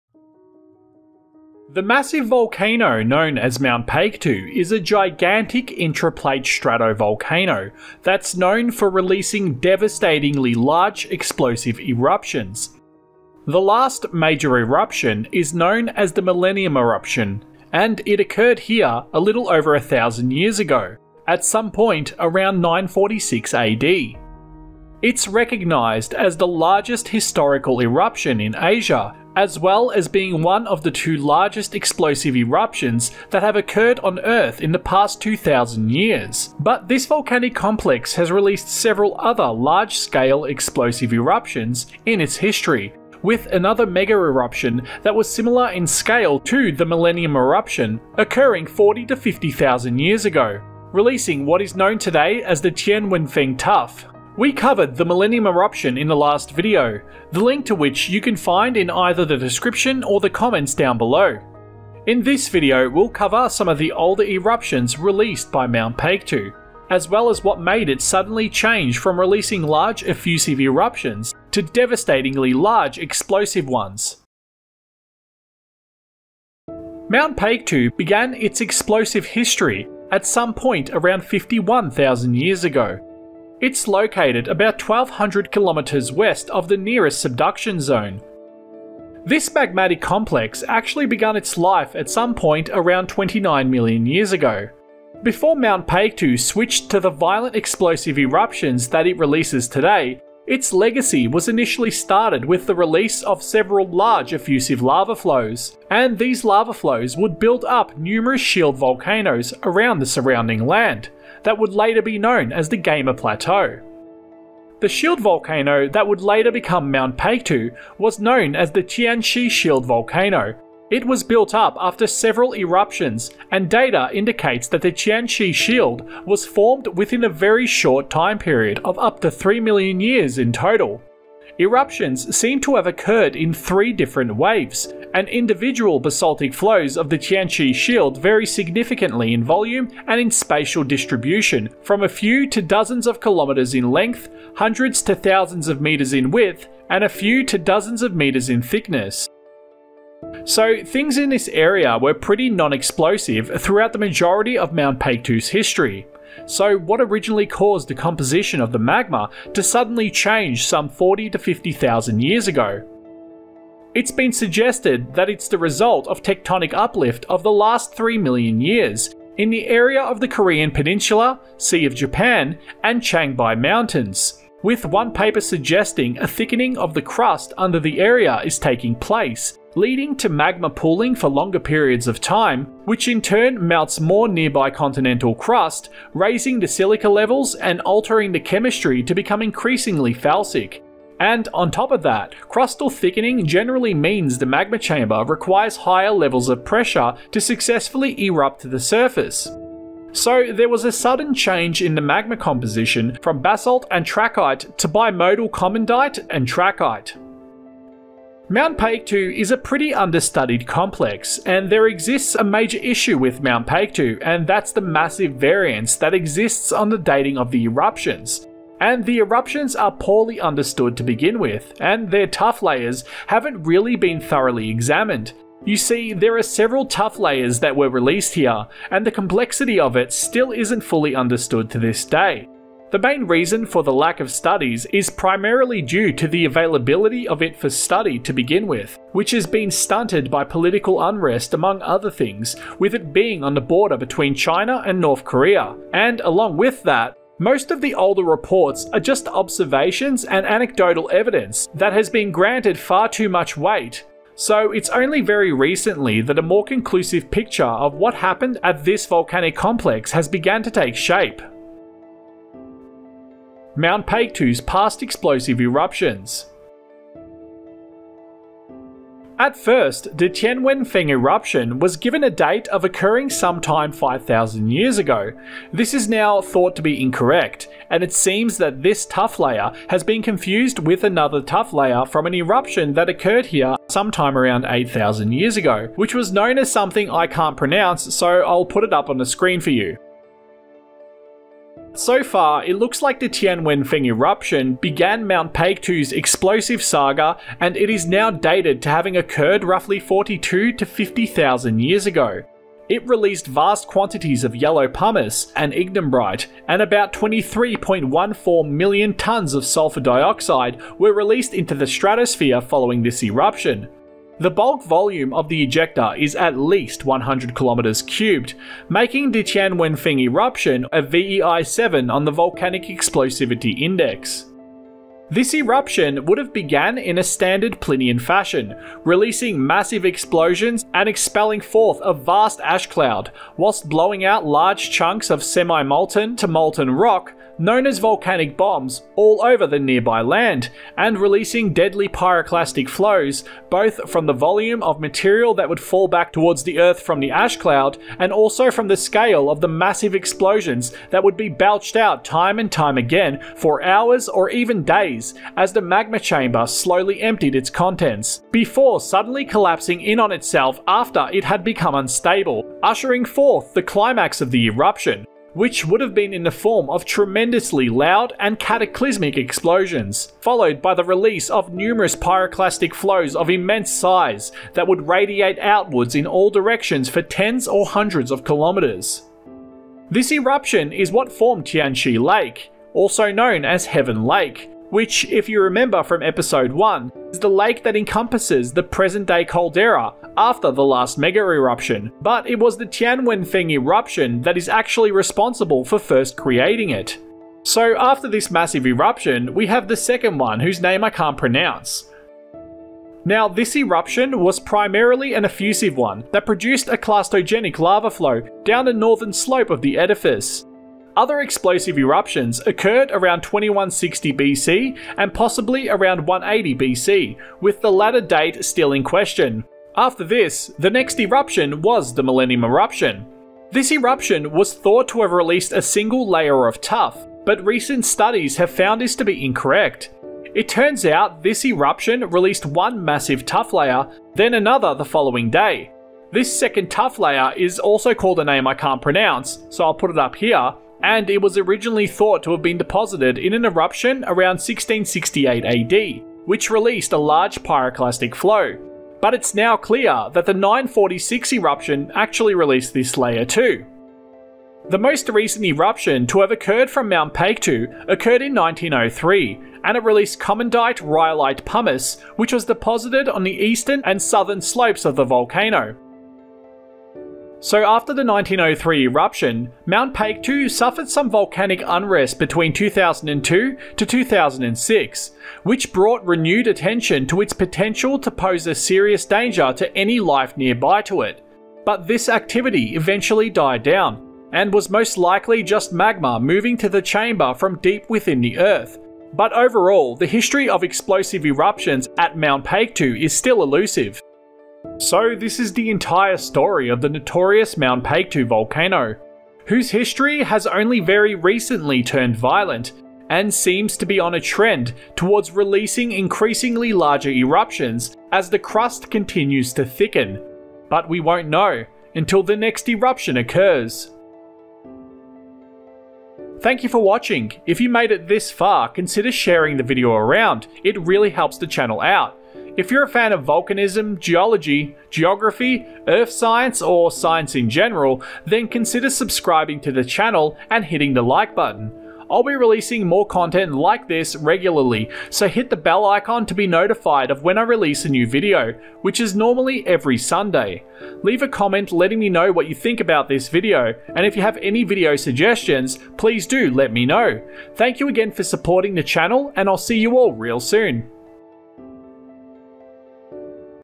Korea's Sleeping Giant: Changbaishan Mountain's Mega Eruption That Could Happen Again!
A ultra plinian volcano located at the North Korean and Chinese owned Paektu mountain, is a stratovolcano that has caused some of the largest eruptions within the past 2000 years, and the largest eruption in mainland China. It's strength has only ever been only being surpassed in recent history by the super volcanic eruption of Lake Taupo in New Zealand. This Super Volcano lies directly on the border of North Korea and China.

The lesser known side of Mount Paektu is it's violent history, and it's origins to life, which we will cover in this documentary.

Mount Paektu goes by several names, Changbaishan volcano is one, along with Baekdu Mountain among others. This volcanic complex begun it's life a little under 30 million years ago, and it released several large effusive eruptions, that spewed forth lava that created the modern day Gaima Plateau. Mount Paektu sits on an ancient shield volcano that was created during these original effusive eruptions, known as The Tianchi Shield. Mount Paektu would remain largely effusive until it suddenly began to grow increasingly violent at some point around fifty thousand years ago. The chemistry of the magma became altered tremendously, and it's thought that this difference might be due to tectonic uplift that is occurring near Mount Paektu, which is creating an environment that fosters the formation of violent eruptions.